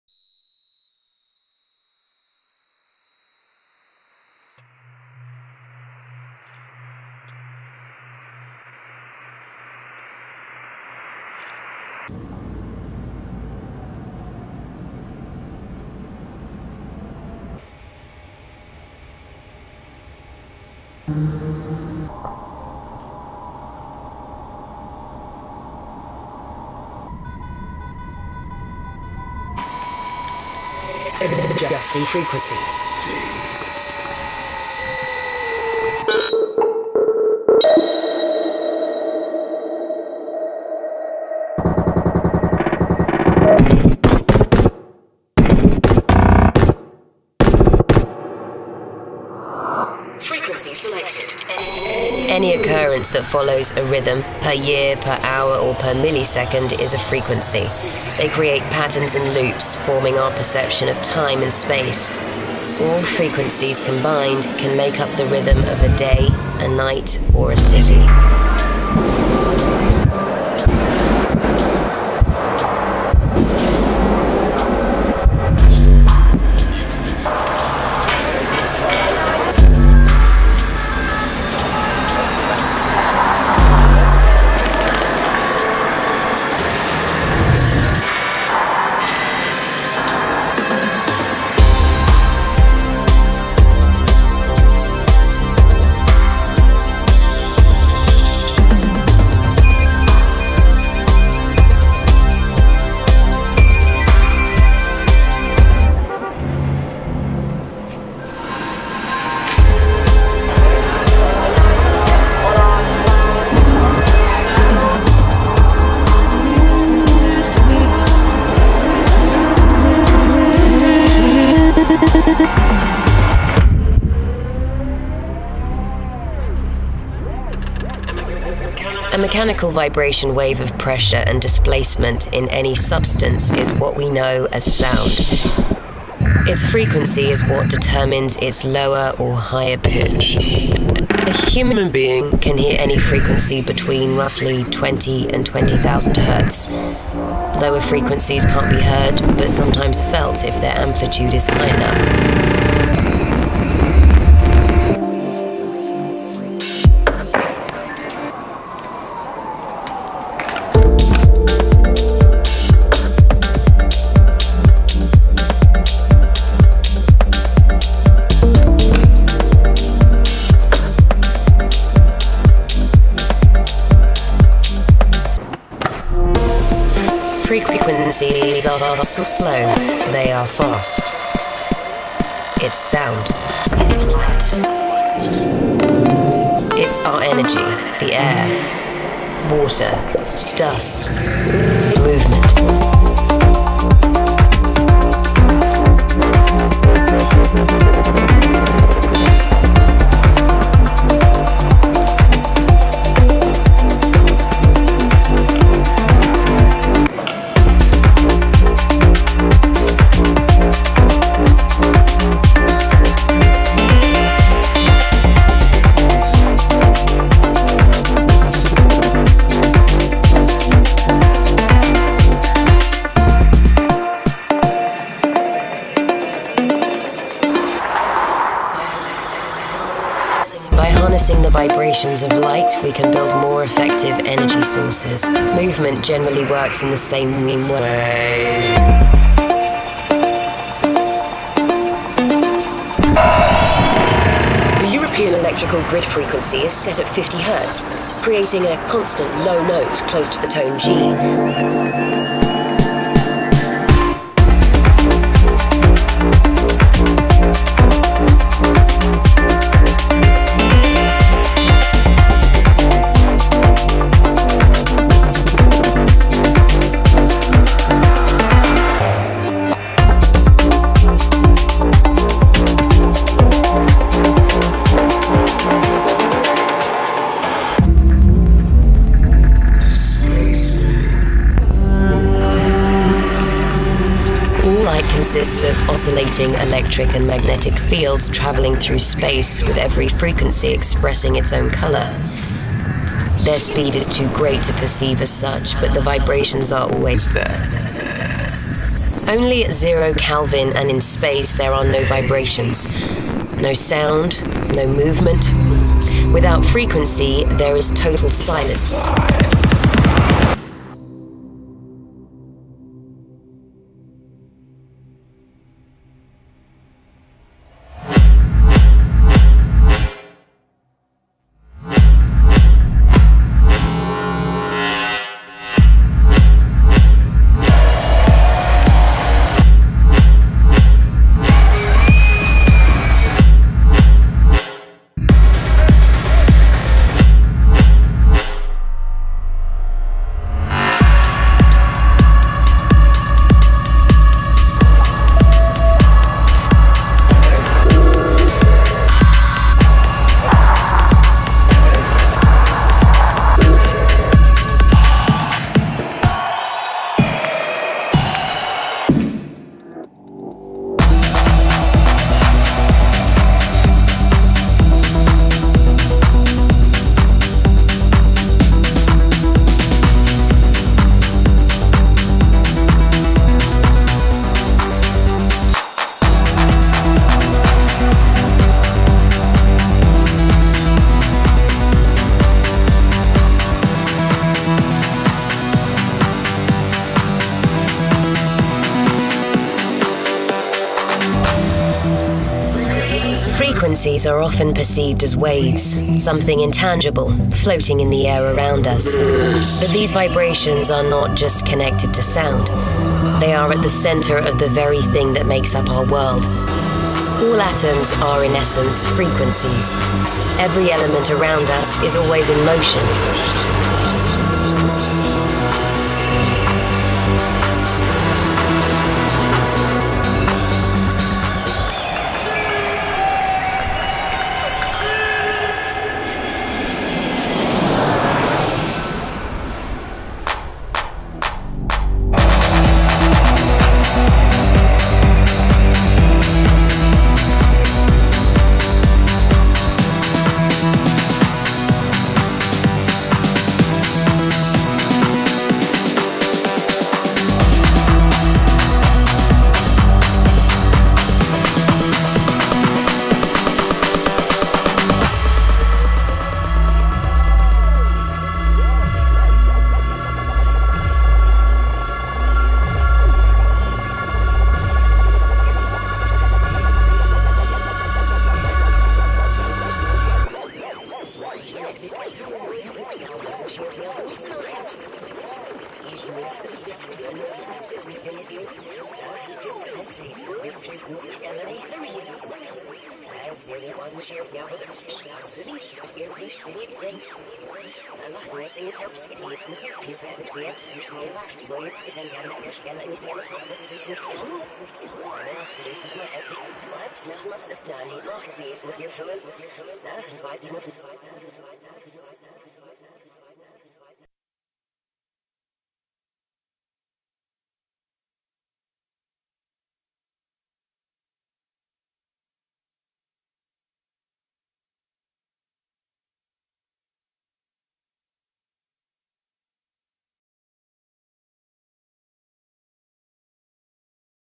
DGTL presents FREQUENCY [Official aftermovie DGTL Festival 2016 - BCN]
Relive DGTL festival 2016 - BCN.

"Barcelona is a city of continuous vibration, where every street and traffic scene is a frequency in itself, pulsating every second of the day.”

This was DGTL Festival 2016 - BCN.
Parc del Forúm, Barcelona

More of DGTL…